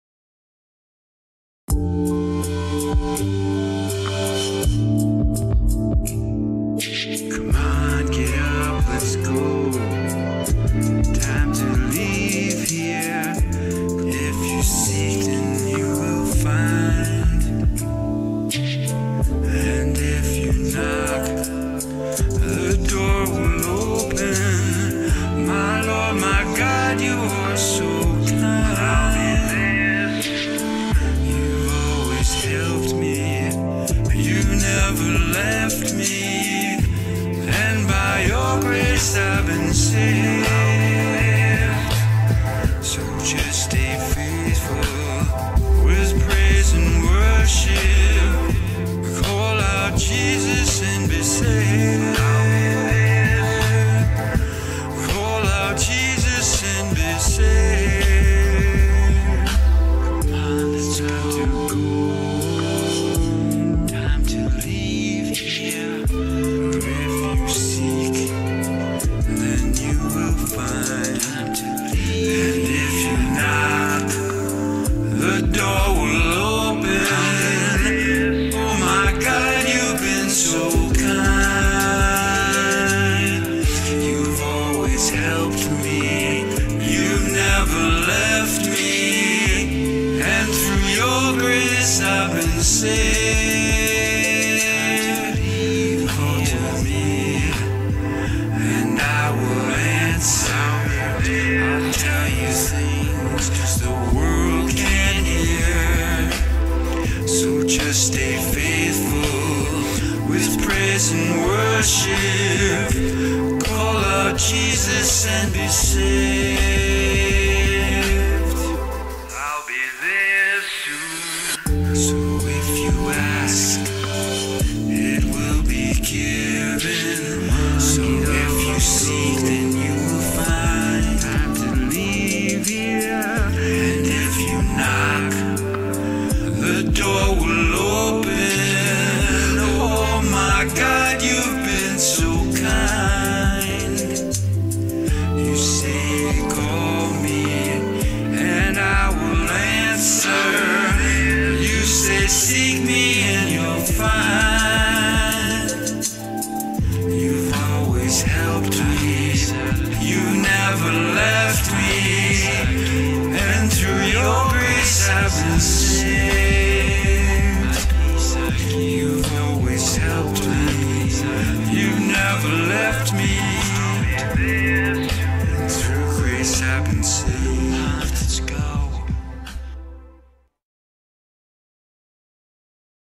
Come On Let's Go
I am definitely feeling a tug on my heart to just go home as I feel like I am just passing through this time and place. Reading "Revelations" aloud over the house tends to do this to me!